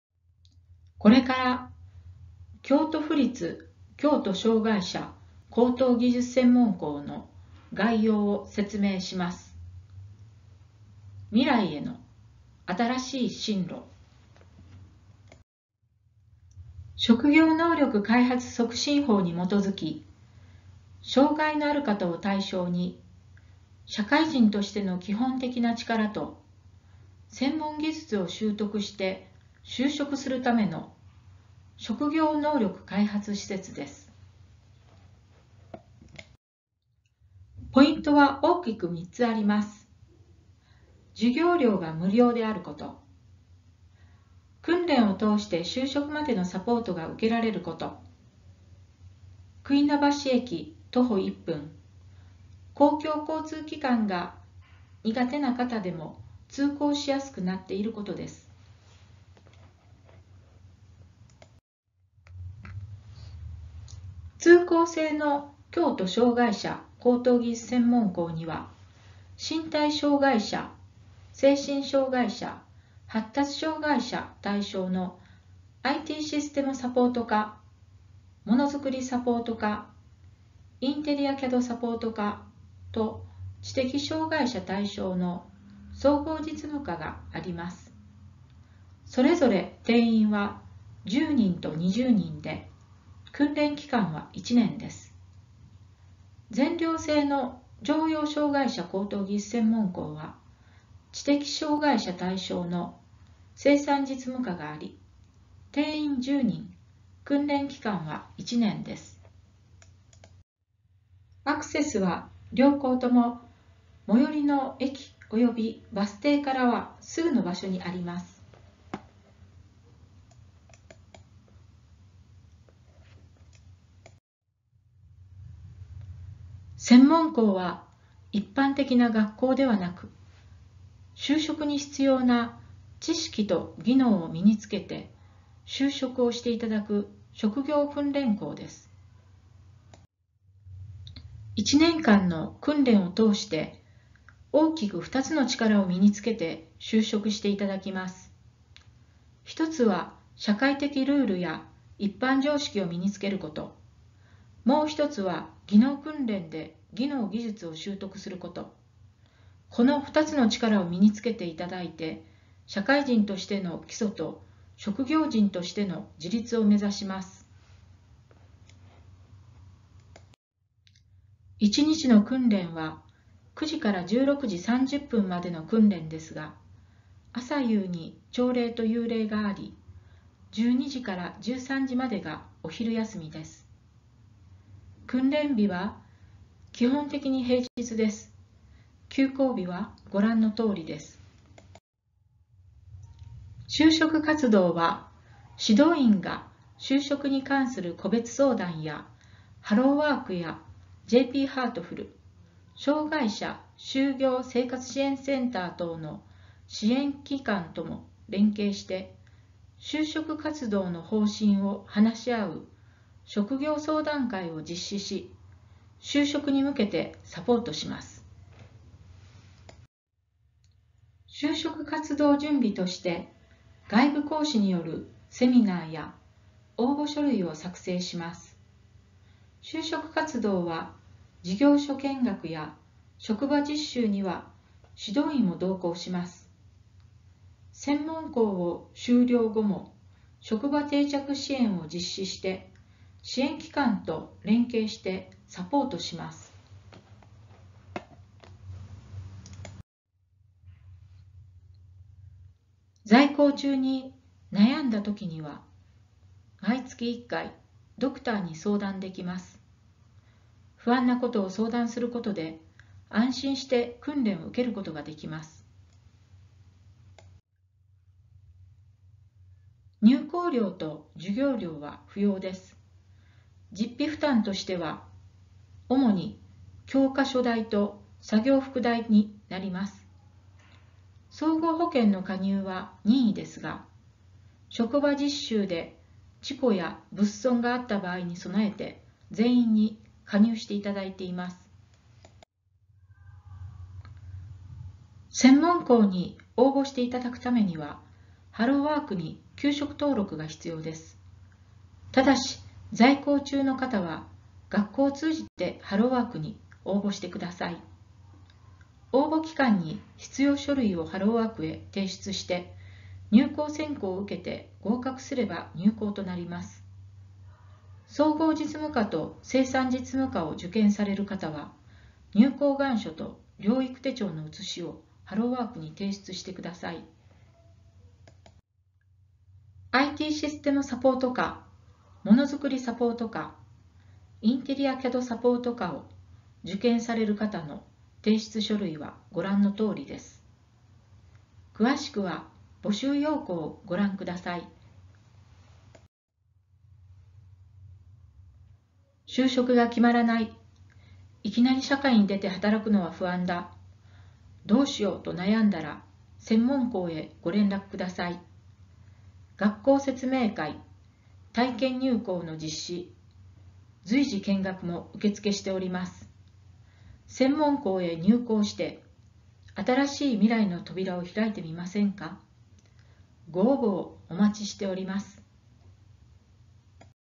京都府立京都障害者高等技術専門校の紹介
京都府立京都障害者高等技術専門校は、職業能力開発促進法（厚生労働省所管）に基づき京都府が設置・運営する障害者を対象とした職業能力開発施設です。